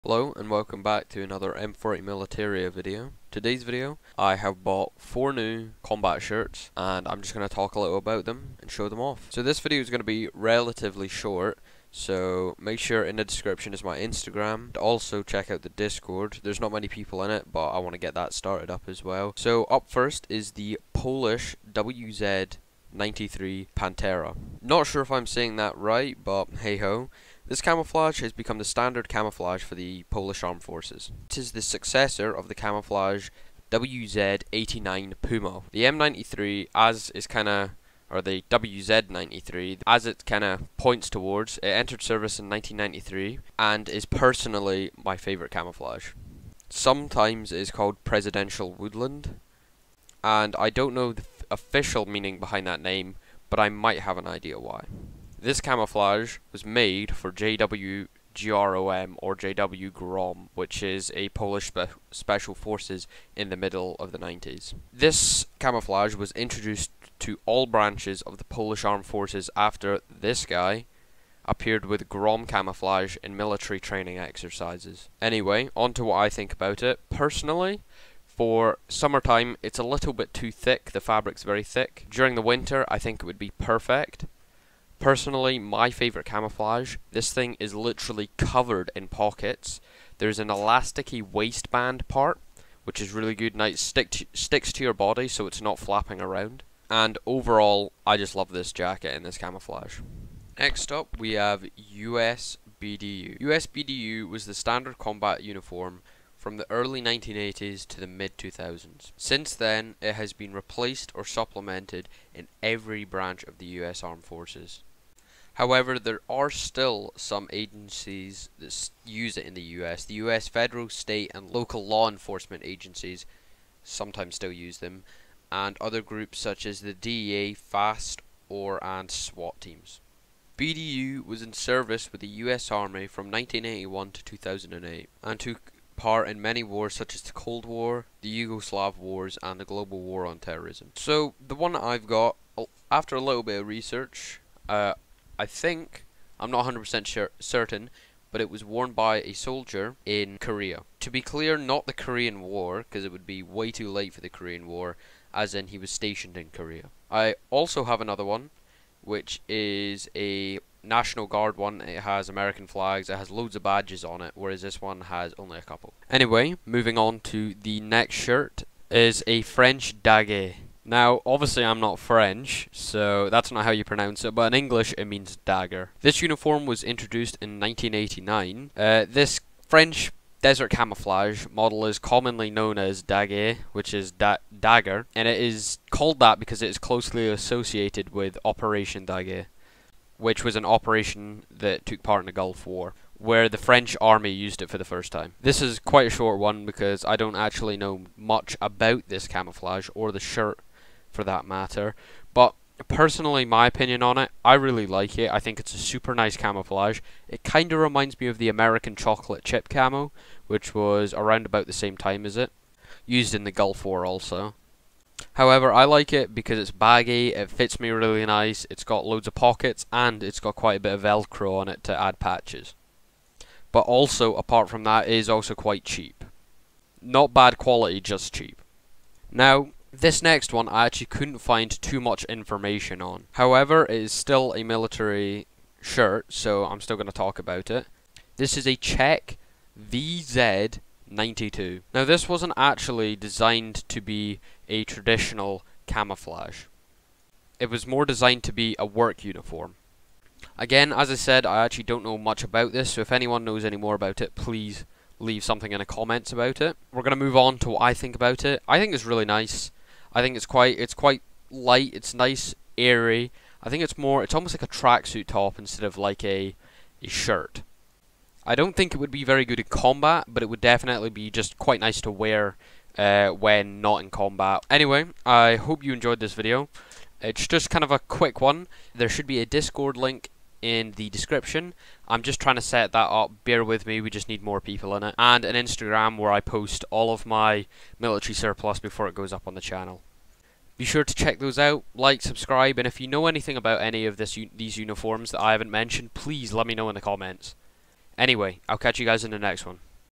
4 New Military Uniforms (Polish Wz. 93) (Czech Vz. 92)(US BDU)(Militaria) (Surplus) (Epic Militaria)
Hope u enjoyed the video please consider liking and subbing, also let me know if I missed any key information out, in the comments or my discord ;)